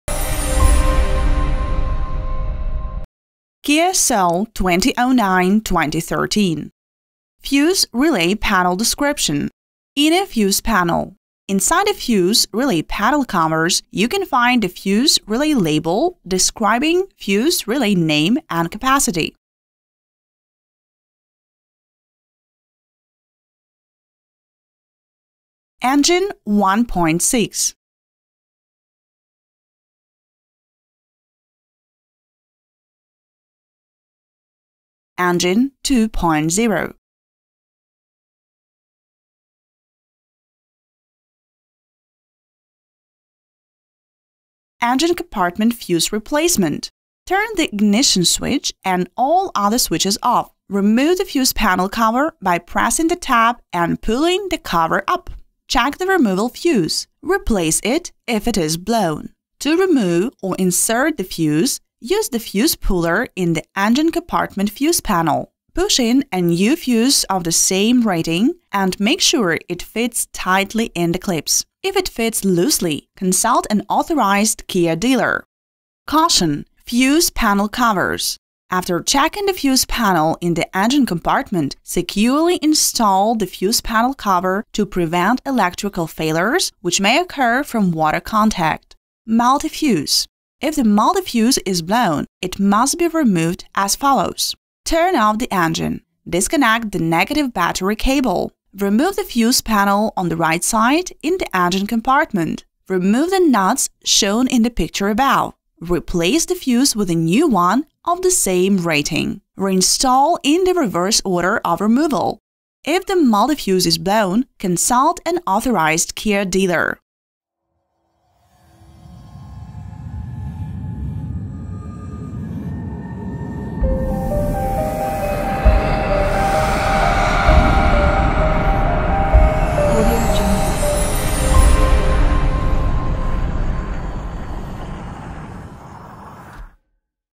Kia Soul 2013 | Fuse and relay panel description | Fuse replacement
Kia Soul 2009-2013 AM Owner's Manual / Maintenance / Fuses
Fuse / relay panel description and Fuse replacement.

0:00 Fuse and relay panel description
0:44 Engine compartment fuse replacement
1:29 Multi fuse

Inner fuse panel

Inside the fuse/relay panel covers, you can find the fuse/relay label describing fuse/relay name and capacity.

Engine compartment fuse replacement

1. Turn the ignition switch and all other switches off.
2. Remove the fuse panel cover by pressing the tab and pulling the cover up.
3. Check the removed fuse; replace it if it is blown. To remove or insert the fuse, use the fuse puller in the engine compartment fuse panel.
4. Push in a new fuse of the same rating, and make sure it fits tightly in the clips. If it fits loosely, consult an authorized Kia dealer.

CAUTION - Fuse panel covers

After checking the fuse panel in the engine compartment, securely install the fuse panel cover to prevent electrical failures which may occur from water contact.

Multi fuse

If the multi fuse is blown, it must be removed as follows:

1. Turn off the engine.
2. Disconnect the negative battery cable.
3. Remove the fuse panel on the right side in the engine compartment.
4. Remove the nuts shown in the picture above.
5. Replace the fuse with a new one of the same rating.
6. Reinstall in the reverse order of removal.

If the multi fuse is blown, consult an authorized Kia dealer.